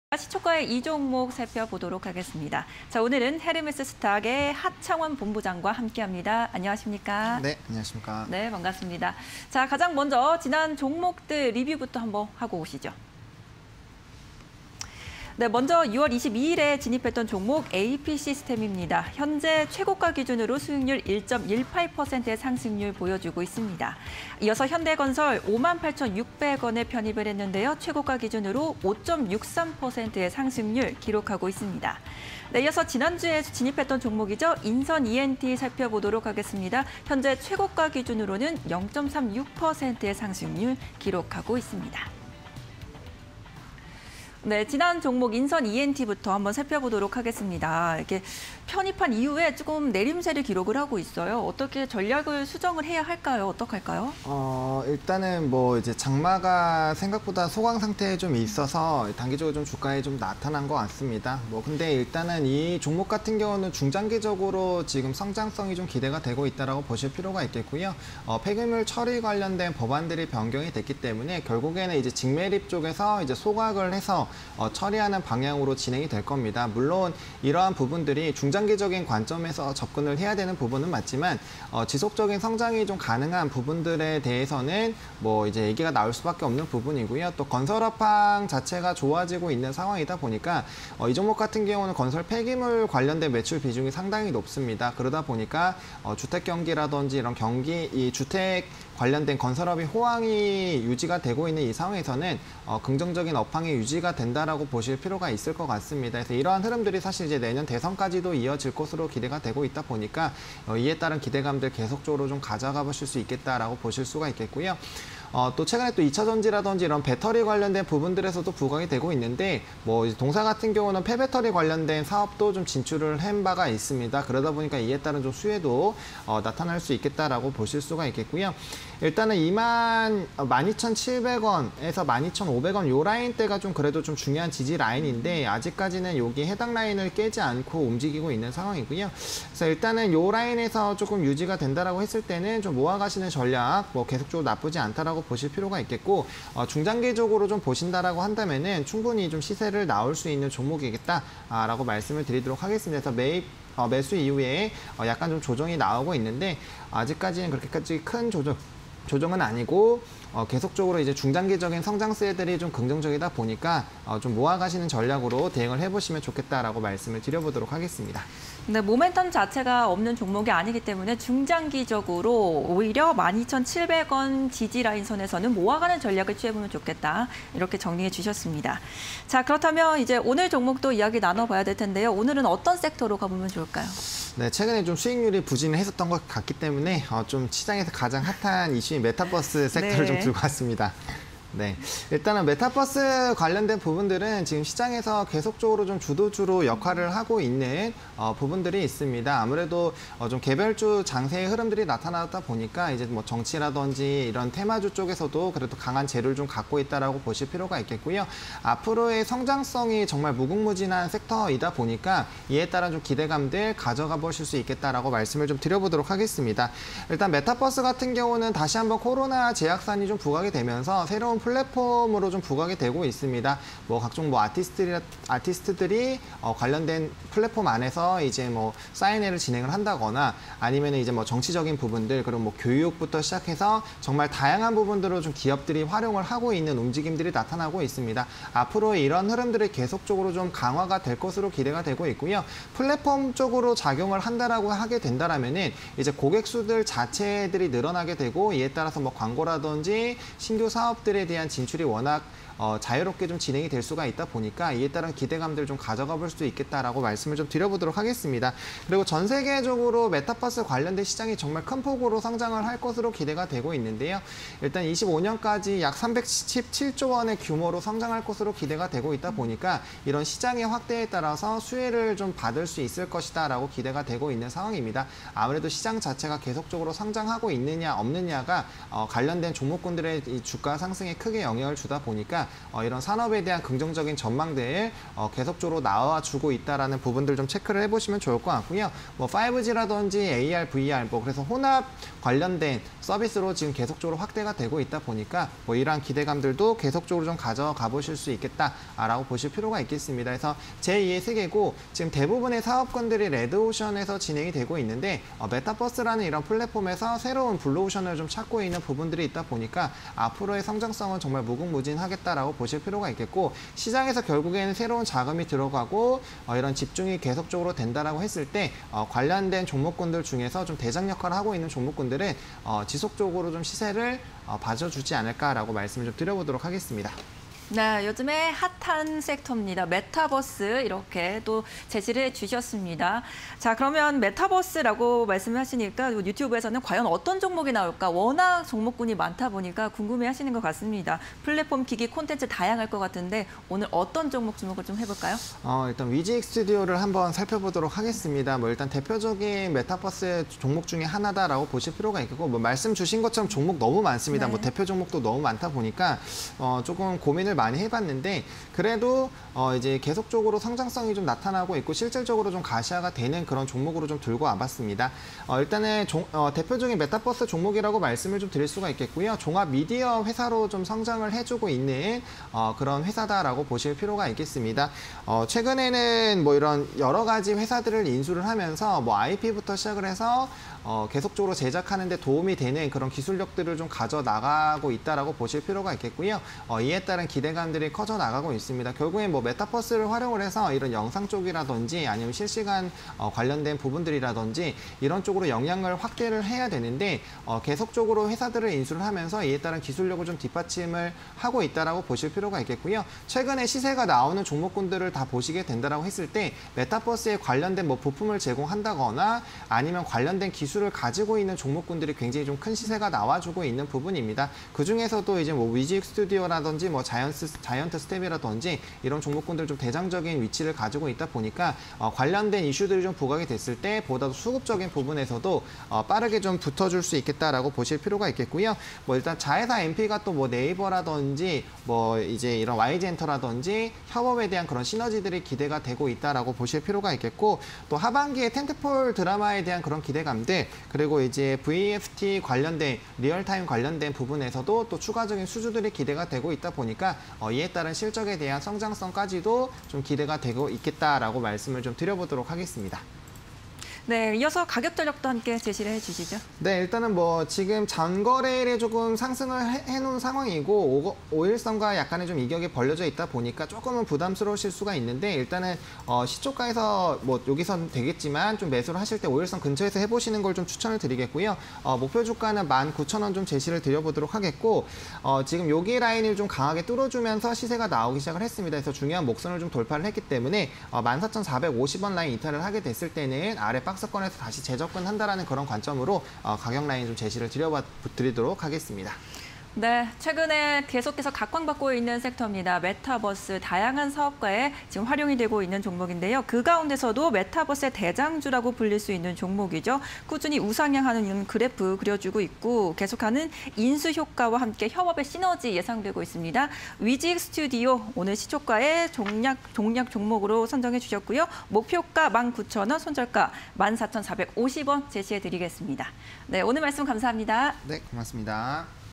위지윅스튜디오(299900)... 메타버스_시초가에 이 종목 (20210713)
- 전 세계 메타버스 시장 급성장 기대
- 5G·AR· VR 기술발전으로 혼합현실 세계 확장
- 다양한 분야에 활용 예정

위지윅스튜디오(299900)
- 대표적 메타버스 관련주
- 종합 미디어 회사로 성장 중
- 자회사와 시너지 효과 기대
- 하반기 텐트폴 드라마 제작 및 VFT 수주 기대
- 래몽레인, NP, 와이랩 등 인수 효과 기대